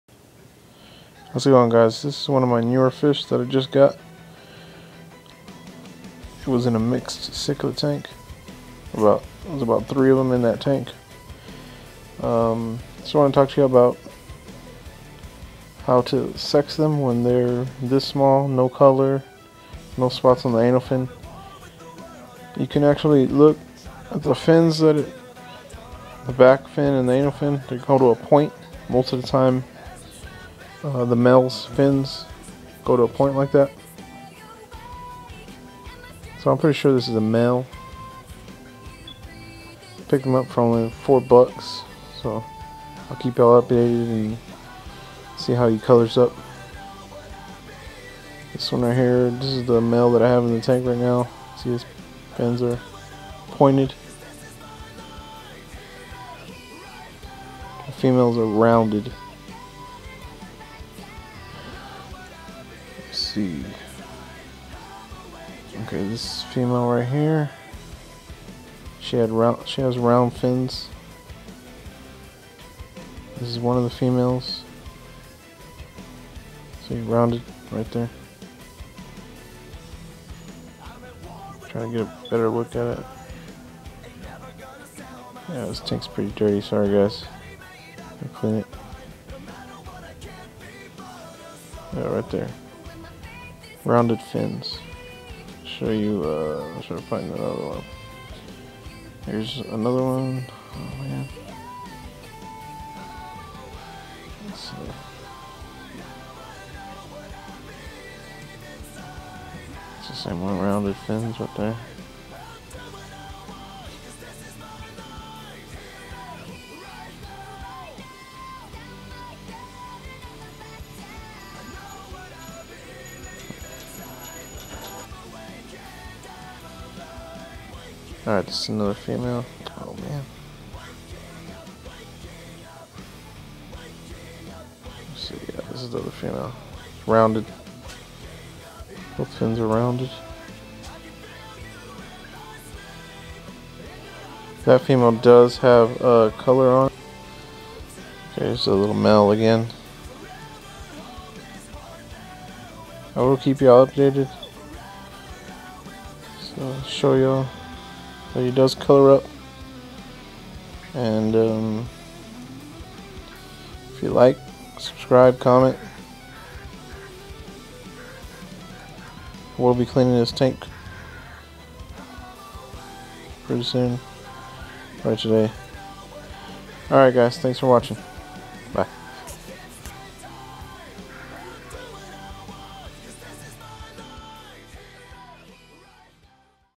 Male or Female African Cichlid
a couple of easy ways to tell if you have a male or female African cichlid